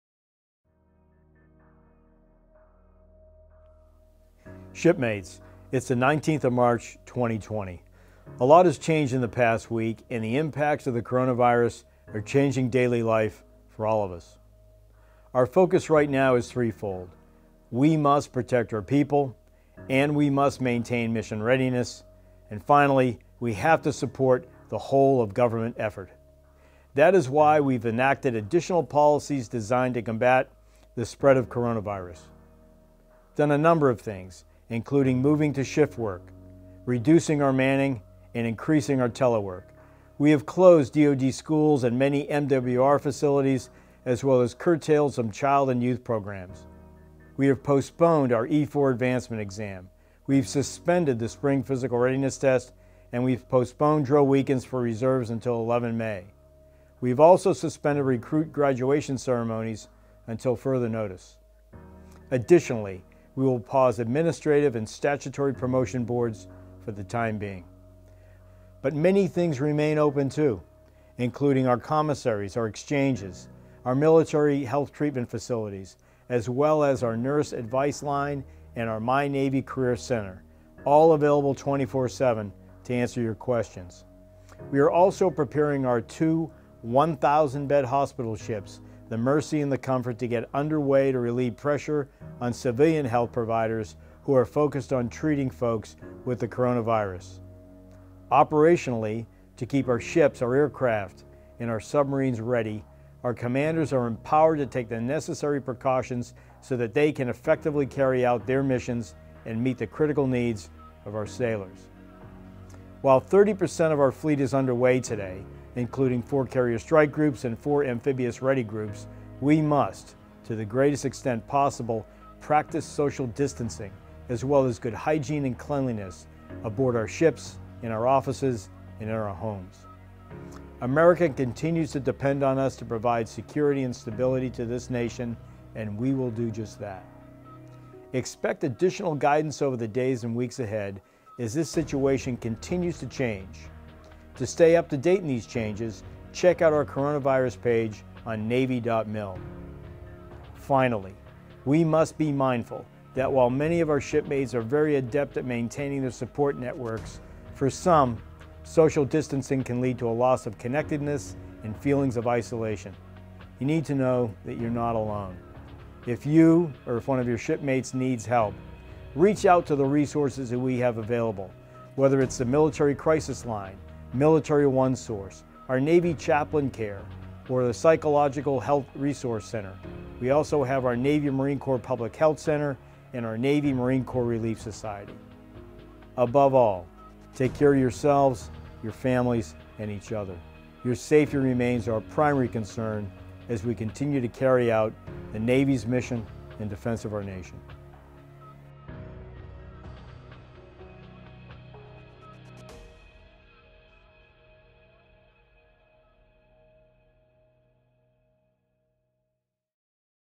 CNO’s Message to the Fleet on Coronavirus
"Shipmates, it’s the 19th of March, 2020, and a lot has changed in the past week. The impacts of the coronavirus are changing daily life for all of us.
Our focus right now is threefold: We must protect our people. We must maintain mission readiness. And finally, we have to support the whole-of-government effort." Chief of Naval Operations, Adm. Mike Gilday.